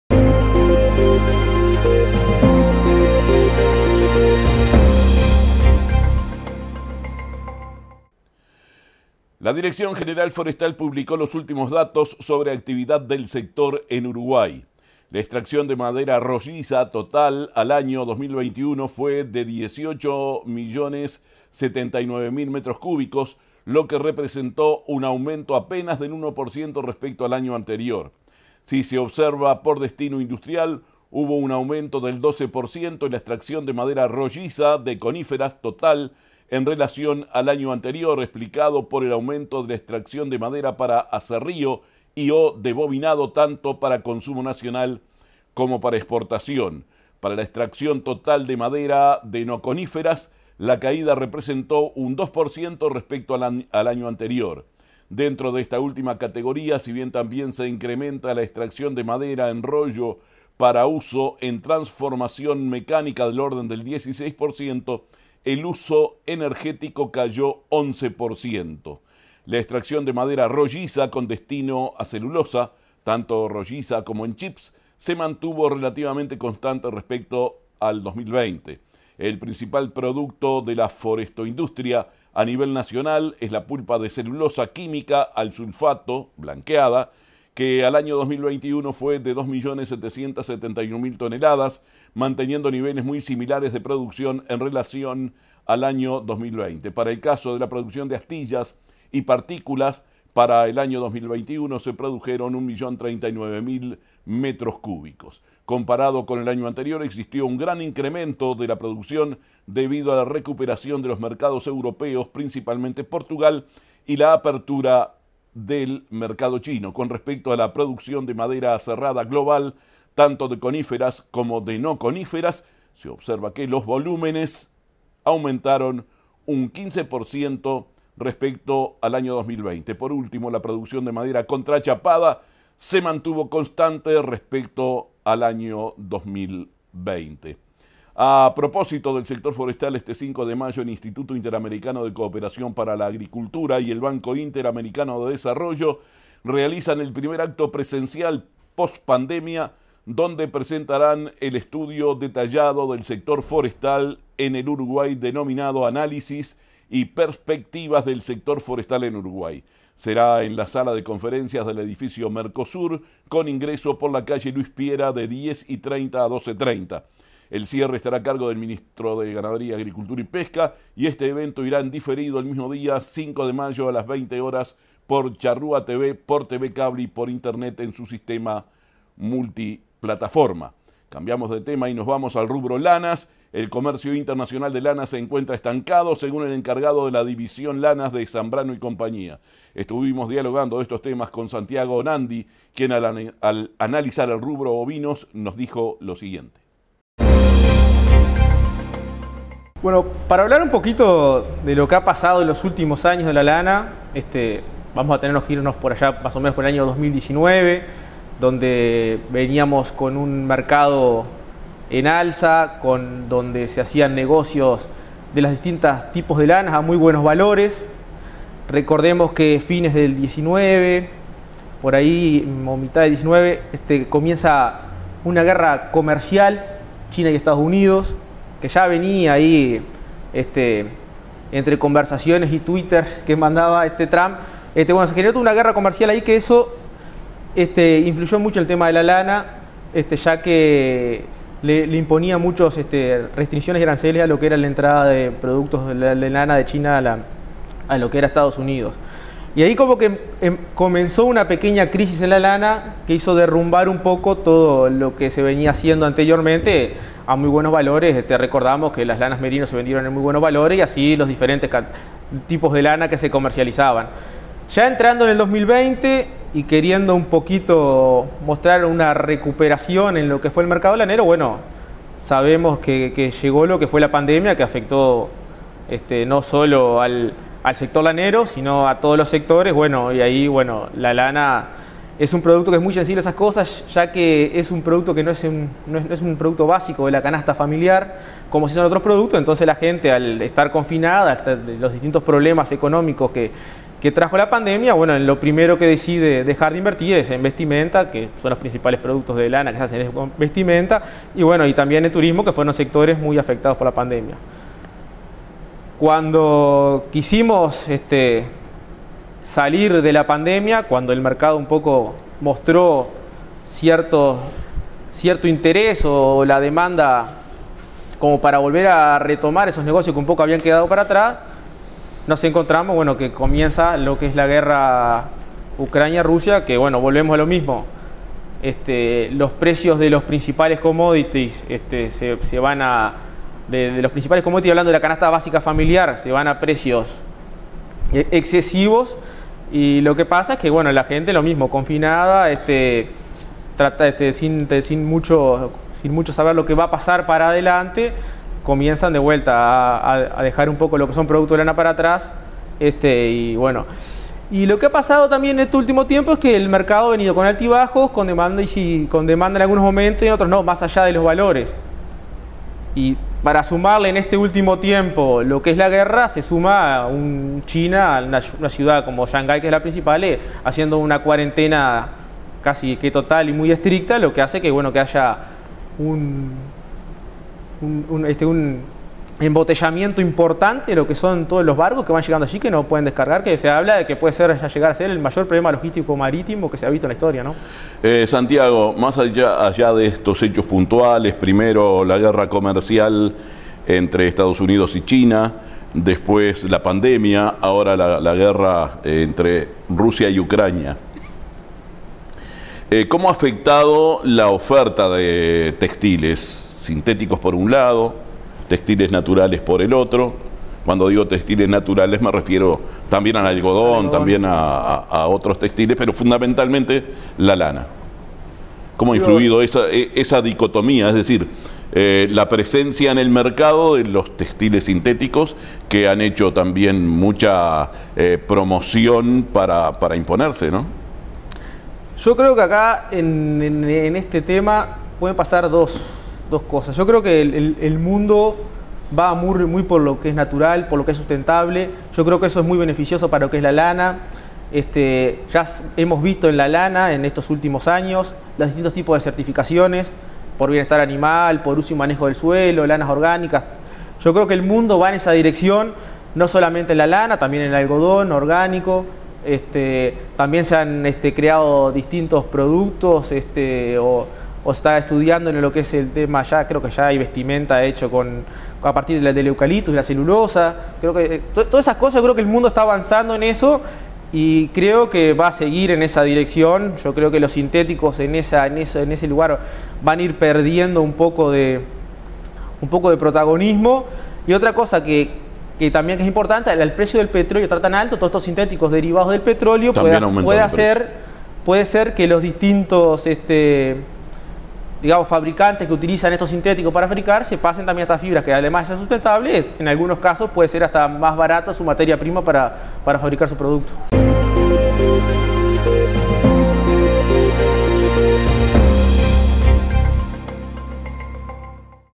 INFORME FORESTAL / SANTIAGO ONANDI - DIVISION LANAS ZAMBRANO & CIA
LA DIRECCIÓN GENERAL FORESTAL PUBLICÓ LOS ÚLTIMOS DATOS SOBRE ACTIVIDAD DEL SECTOR EN URUGUAY.

LA EXTRACCIÓN DE MADERA ROLLIZA TOTAL AL AÑO 2021 FUE DE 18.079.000 METROS CÚBICOS, LO QUE REPRESENTÓ UN AUMENTO APENAS DEL 1% RESPECTO AL AÑO ANTERIOR.
SI SE OBSERVA POR DESTINO INDUSTRIAL, HUBO UN AUMENTO DEL 12% EN LA EXTRACCIÓN DE MADERA ROLLIZA DE CONÍFERAS TOTAL EN RELACIÓN AL AÑO ANTERIOR, EXPLICADO POR EL AUMENTO DE LA EXTRACCIÓN DE MADERA PARA ASERRÍO Y/O DE BOBINADO TANTO PARA CONSUMO NACIONAL COMO PARA EXPORTACIÓN.

PARA LA EXTRACCIÓN TOTAL DE MADERA DE NO CONÍFERAS, LA CAÍDA REPRESENTÓ UN 2% RESPECTO AL AÑO ANTERIOR.

DENTRO DE ÉSTA ÚLTIMA
CATEGORÍA, SI BIEN TAMBIÉN SE INCREMENTA LA EXTRACCIÓN DE MADERA EN ROLLO PARA USO EN TRANSFORMACIÓN MECÁNICA DEL ORDEN DEL 16%, EL USO ENERGÉTICO CAYÓ UN 11%.
LA EXTRACCIÓN DE MADERA ROLLIZA CON DESTINO A CELULOSA (TANTO ROLLIZA COMO EN CHIPS) SE MANTUVO RELATIVAMENTE CONSTANTE RESPECTO AL AÑO 2020.
LA PUBLICACION QUE CONTIENE ESTOS DATOS, HA SIDO ELABORADA CON DATOS GENERADOS EN DIRECCION GENERAL FORESTAL Y DE OTROS ORGANISMOS, INSTITUCIONES Y EMPRESAS VINCULADAS AL SECTOR Y PROCESADA POR PERSONAL TÉCNICO DE LA MENCIONADA DIVISIÓN.
EL PRINCIPAL PRODUCTO DE LA FORESTO-INDUSTRIA A NIVEL NACIONAL ES LA PULPA DE CELULOSA QUÍMICA AL SULFATO (BLANQUEDA) QUE AL AÑO 2021 FUE DE 2,771.000 TONELADAS, MANTENIENDO NIVELES MUY SIMILARES DE PRODUCCIÓN EN RELACIÓN AL AÑO 2020.
PARA EL CASO DE LA PRODUCCIÓN DE ASTILLAS Y PARTÍCULAS PARA EL AÑO 2021 SE PRODUJERON 1,039.000 METROS CÚBICOS.

COMPARADO CON EL AÑO ANTERIOR, EXISTIÓ UN GRAN INCREMENTO DE LA
PRODUCCIÓN DEBIDO A LA RECUPERACIÓN DE LOS MERCADOS EUROPEOS (PRINCIPALEMENTE PORTUGAL) Y APERTURA DEL MERCADO CHINO.

CON RESPECTO A LA PRODUCCIÓN DE MADERA ASERRADA GLOBAL (TANTO DE CONÍFERAS COMO DE NO CONÍFERAS), SE OBSERVA QUE LOS VOLUMENES AUMENTARON UN 15% RESPECTO AL AÑO 2020.

POR ÚLTIMO, LA PRODUCCIÓN DE MADERA CONTRACHAPADA SE MANTUVO CONSTANTE RESPECTO AL AÑO 2020.

A PROPÓSITO DEL SECTOR FORESTAL ESTE 5 DE MAYO EL INSTITUTO INTERAMERICANO DE COOPERCION PARA LA AGRICULTURA Y EL BANCO INTERAMERICANO DE DESARROLLO, REALIZAN EL PRIMER ACTO PRESENCIAL POS PANDEMIA, DONDE  PRESENTARÁN EL ESTUDIO DETALLADO DEL SECTOR FORESTAL EN EL URUGUAY DENOMINADO “ANÁLISIS Y PERSPECTIVAS DEL SECTOR FORESTAL EN URUGUAY”
SERÁ EN LA SALA DE CONFERENCIAS DEL EDIFICIO MERCOSUR CON INGRESO POR LA CALLE LUIS PIERA DE 10 Y 30 A 12 Y 30.
EL CIERRE ESTARÁ A CARGO DEL MINISTRO DE GANADERÍA AGRICULTURA Y PESCA Y ESTE EVENTO IRÁ EN DIFERIDO EL MISMO DIA 5 DE MAYO A LAS 20 HORAS POR CHARRUATV POR TV CABLE Y POR INTERNET EN SUS SISTEMA MULTIPLATAFORMA.

EL COMERCIO INTERNACIONAL DE LANAS SE ENCUENTRA ESTANCADO, SEGÚN EL ENCARGADO DE LA DIVISION LANAS DE ZAMBRANO Y COMPAÑÍA.
ESTUVIMOS DIALOGANDO CON SANTIAGO ONANDI QUIEN AL ANALIZAR EL RUBRO